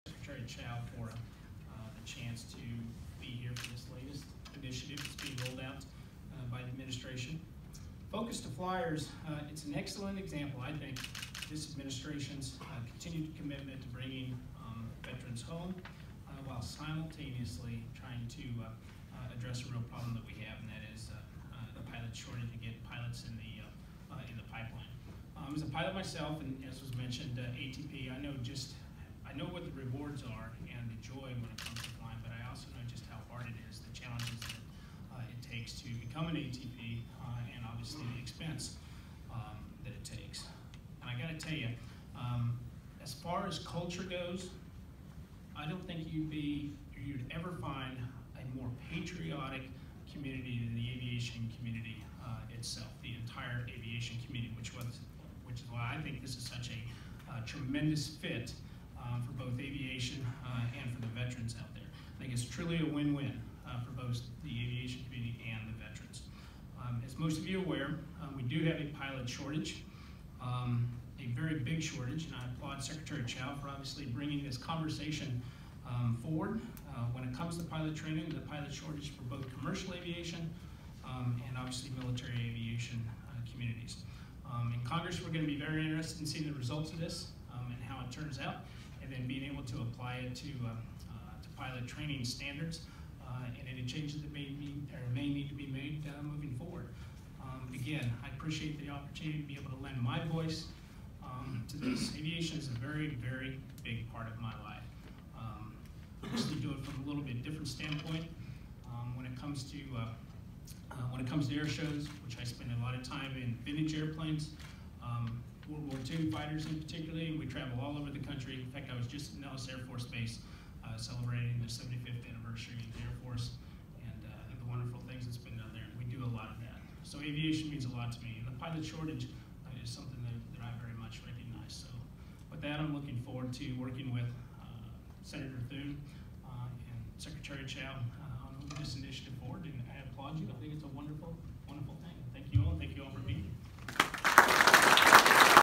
DOT: Forces To Flyers
Congressman Sam Graves joined U.S. Secretary of Transportation Elaine L. Chao to announced the creation of Forces to Flyers, a new research initiative aimed at supporting military veterans who are interested in becoming commercial pilots.